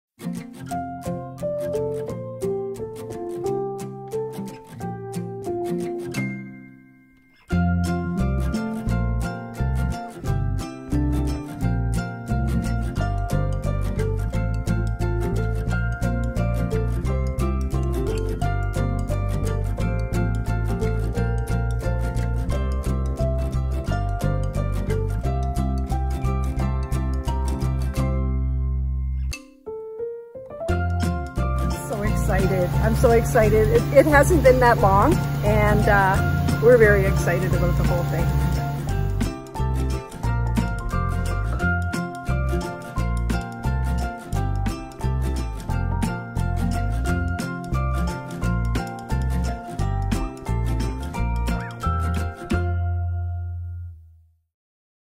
Mary’s Place
Getting ready to set a custom prefab home on Feb. 25, 2021